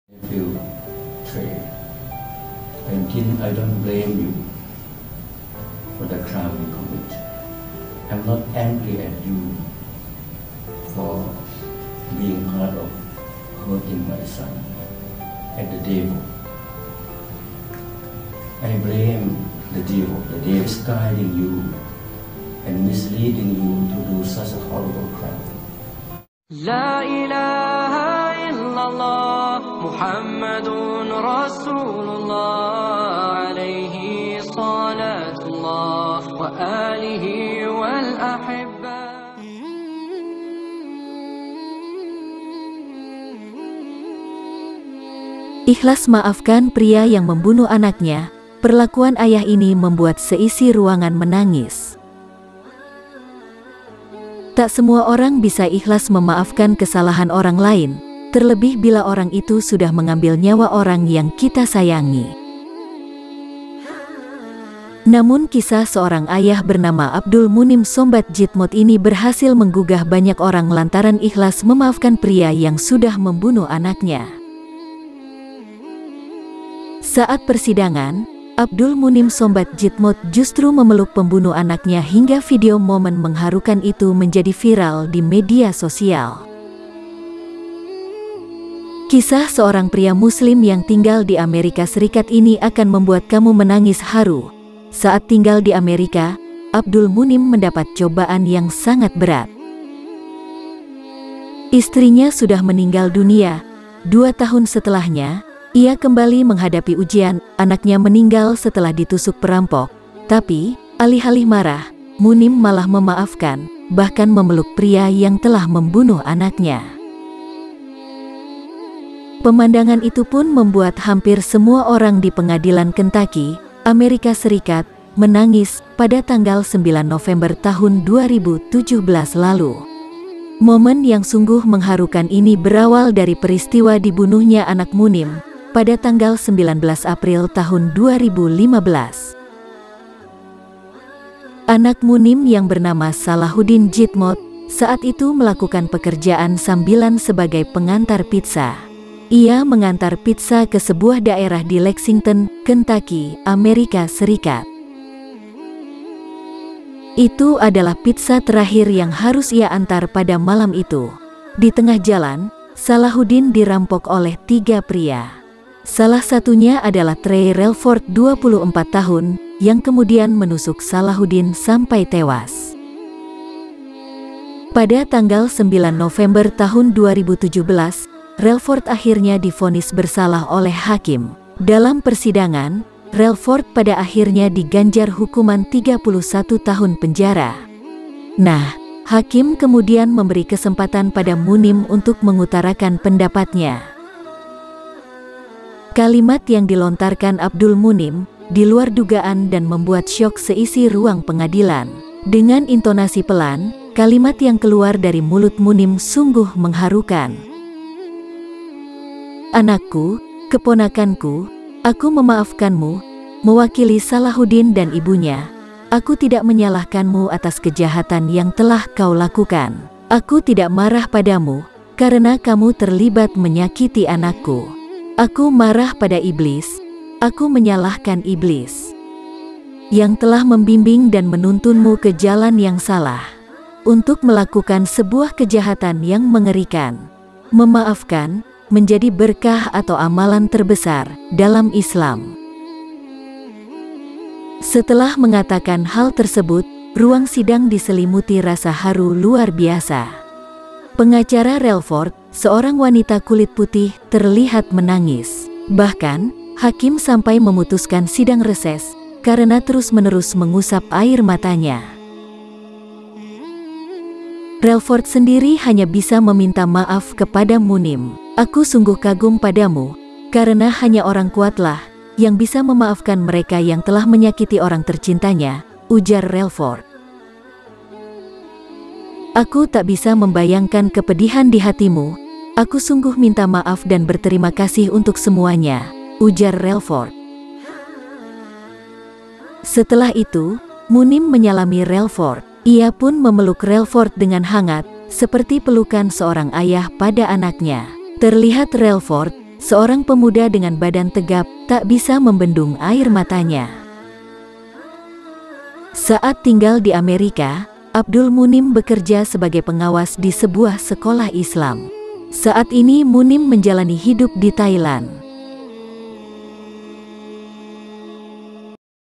Orang Ramai Belajar ISLAM Karena Lihat Video Ini | Kisah Mengharukan Abdul Munim Sombat Jitmoud
Ikhlas Maafkan Pria yang Membunuh Anaknya, Perlakuan Ayah ini Membuat Seisi Ruangan Menangis

Tak semua orang bisa ikhlas memaafkan kesalahan orang lain, terlebih bila orang itu sudah mengambil nyawa orang yang kita sayangi.

Namun kisah seorang ayah bernama Abdul Munim Sombat Jitmoud ini berhasil menggugah banyak orang lantaran ikhlas memaafkan pria yang sudah membunuh anaknya.

Kisah seorang pria muslim yang tinggal di Amerika Serikat ini akan membuat kamu menangis haru.
Saat tinggal di Amerika, Abdul Munim mendapat cobaan yang sangat berat.